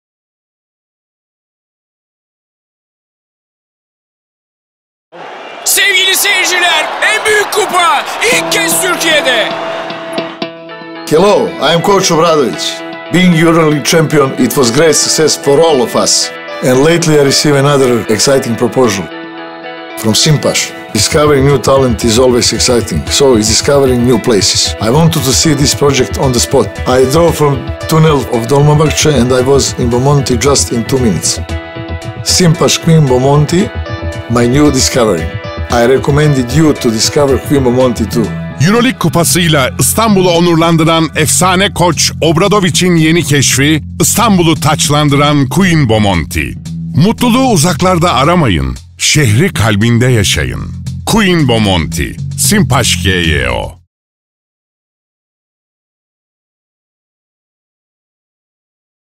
Obradoviç'in yeni reklam filmi
Sinpaş GYO, Zejko Obradoviç’le reklam filmi çekimlerini Queen Bomonti’deki örnek evlerinde gerçekleştirdi.